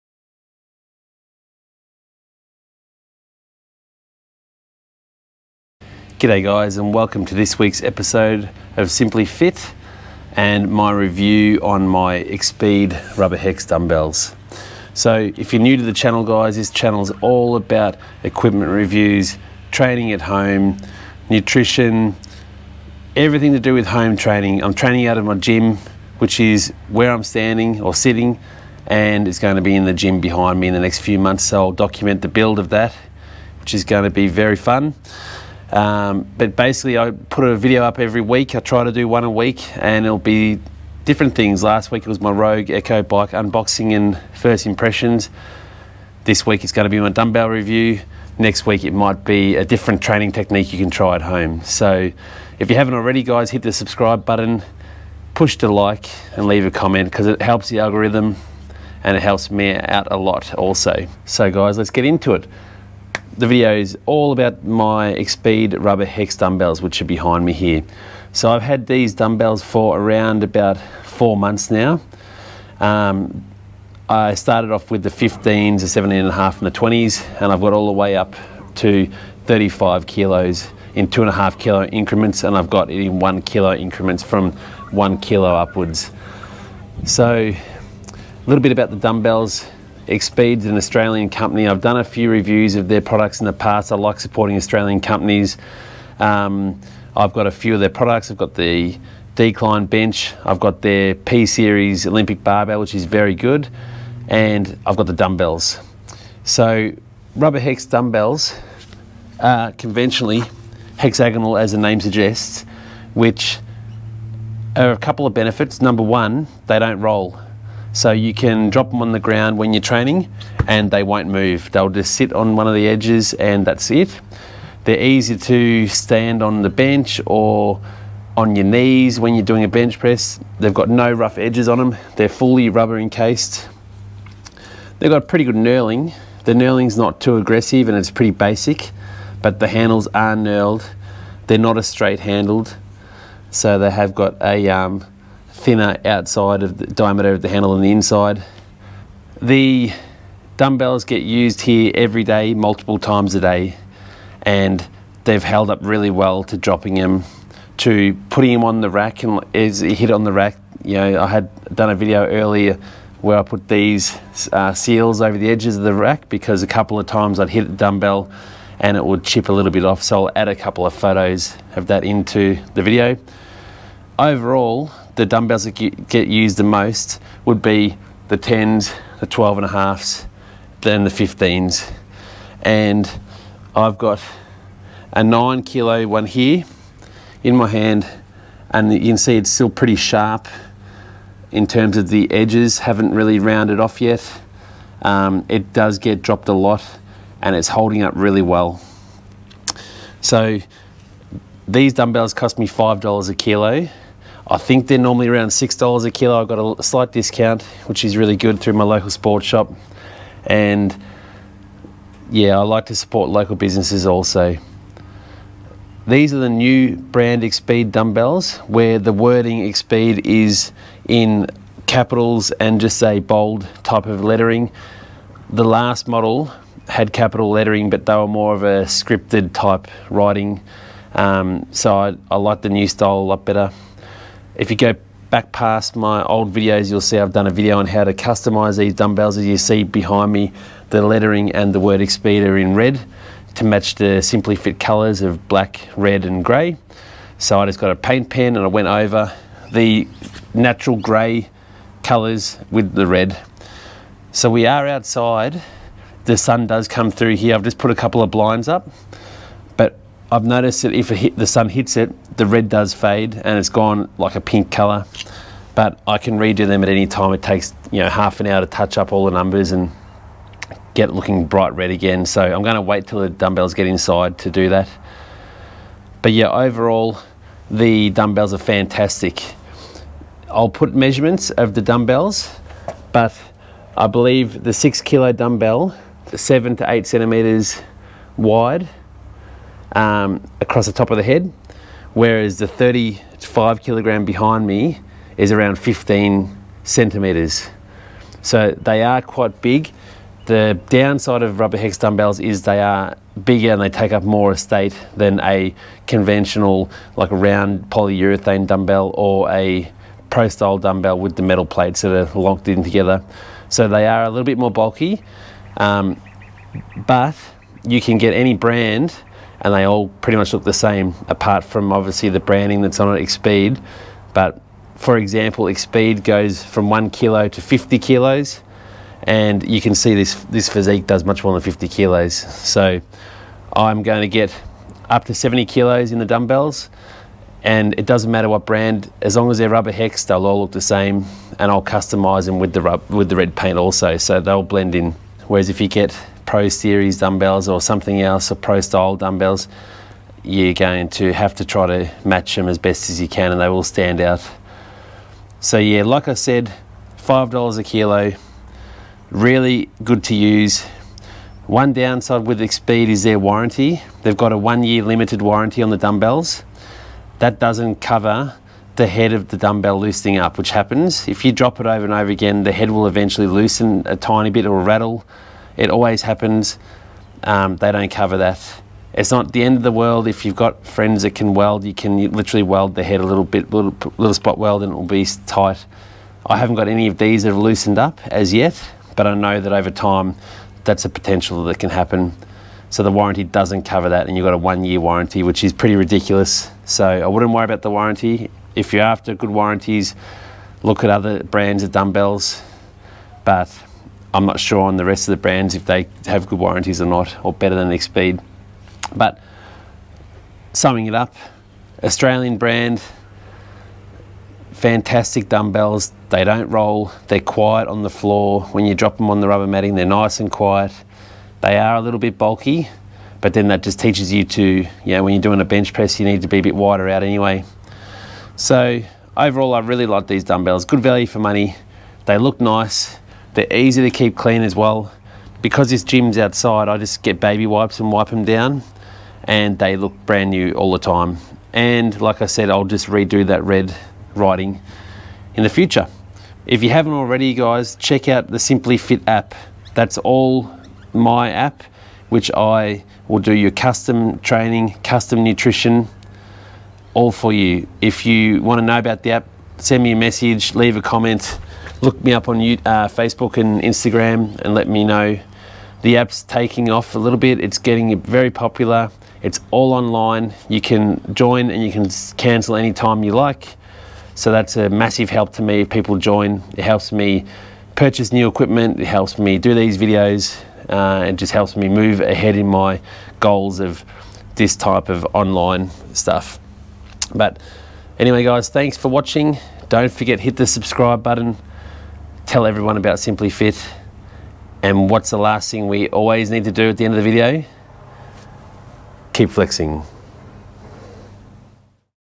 Xpeed Dumbbell Review
My first review on the Xpeed Rubber Hex Dumbbells.

Rubber hex dumbbells are fantastic as they are hard wearing, look god, won't roll around and every brand looks the same. Xpeed come in 1-50kg dumbbell pairs and are priced any $5.95 per kilo.